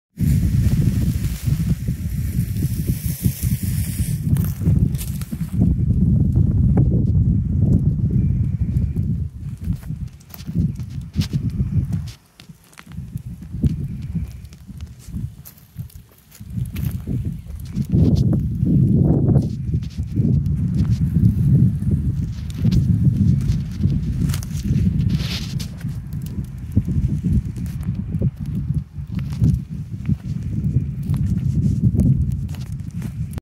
Old mesquite tree
Burning a mesquite tree that is taking water from an old old Cottonwood tree.